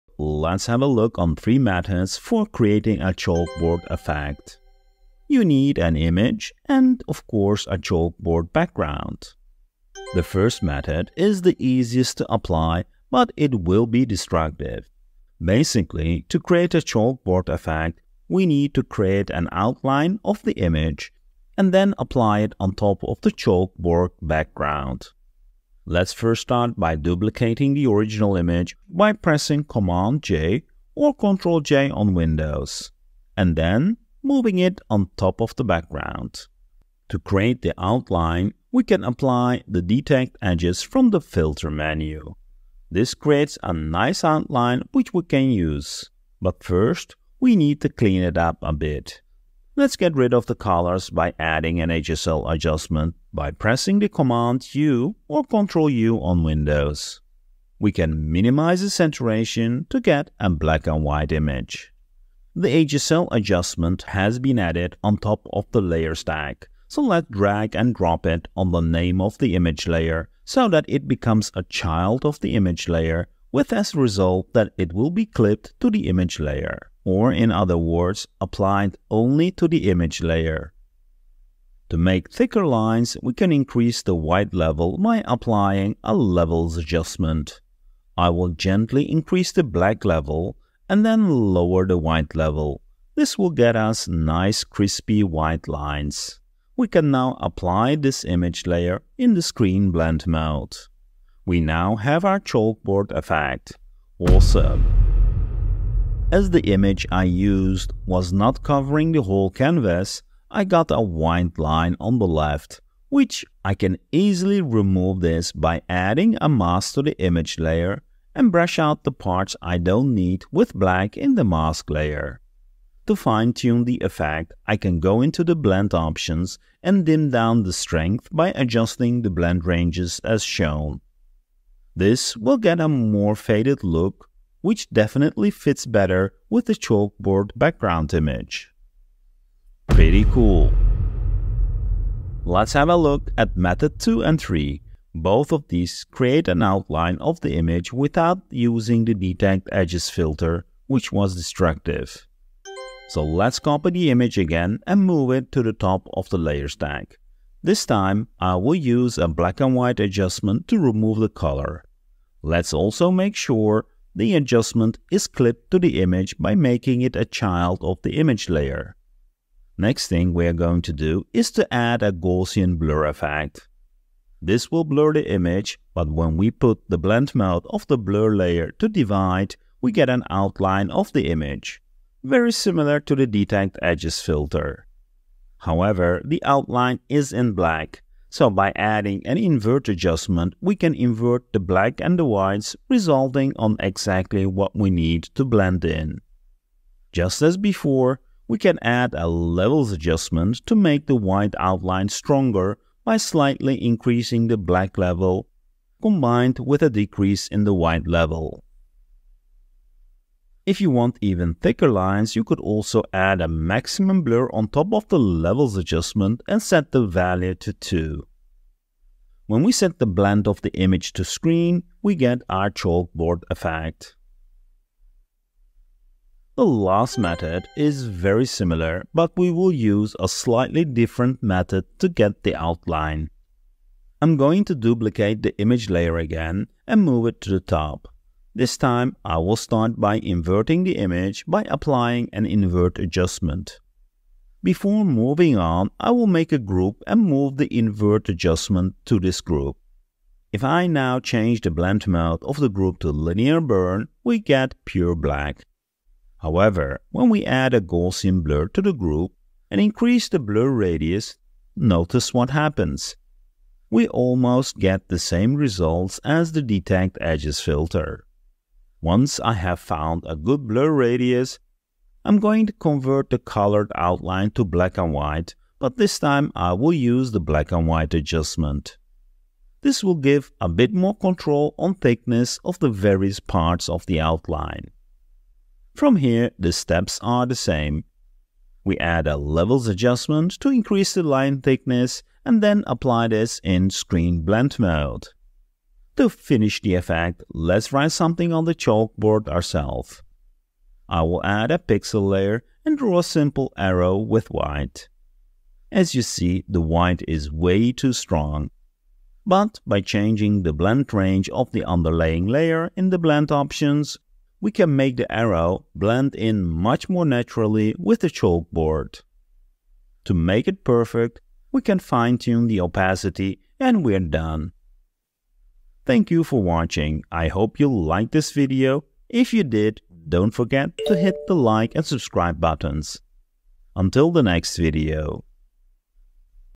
How to blend an image on a chalkboard using Affinity Photo (3 methods)
In this video, i will show 3 methods on creating a chalkboard effect. The idea is to get the outlines in an image in white and then blend this in with the chalkboard so that it looks like it has been drawn with chalk.

After creating the basic effect, i will share small tips to make the blend look more realistic.